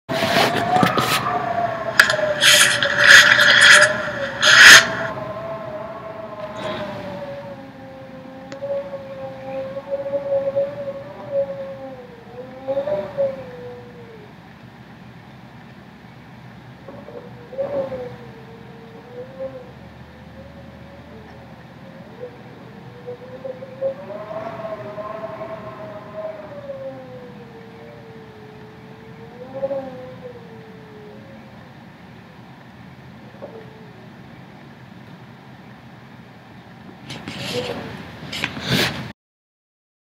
Las Vegas at Night
The sound inside my hotel room